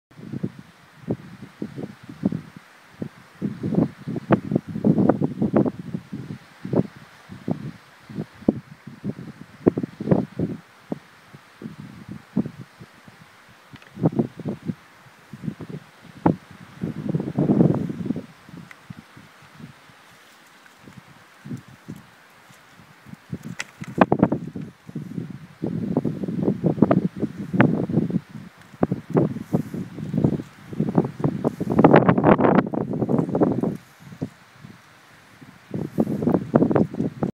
2 pucks fighting
Saw this today on my return journey from Carrauntuohill mountain. lasted about ten minutes but was only able to capture this amount. Glad to say...both pucks settled their differences and went their separate ways